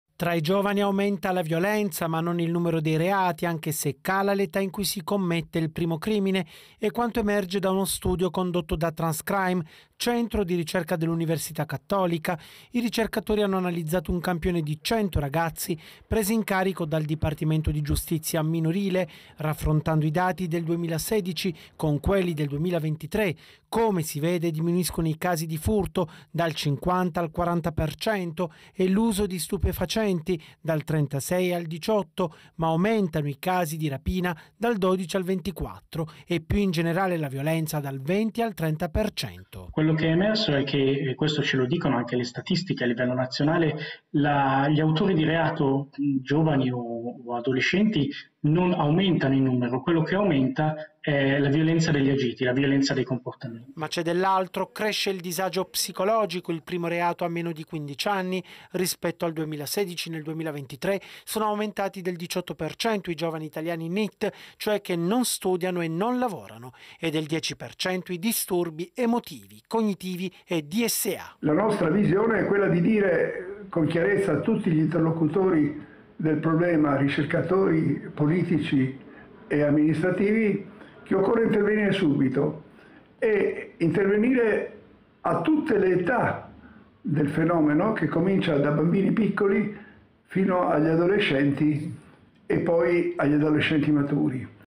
Giovani più violenti ma non cresce il numero dei reati
Aumenta tra i giovani la natura violenta dei reati commessi da chi delinque e cala l'età in cui si commette il primo reato, ma il numero dei casi non cresce.